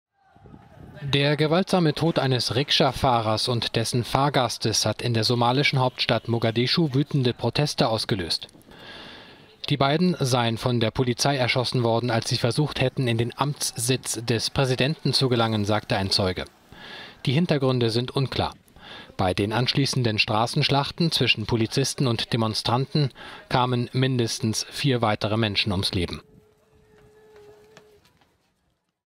Somalia: Tod eines Rikscha-Fahrers löst Straßenschlacht aus
Mindestens sechs Menschen kamen in Mogadischu ums Leben.…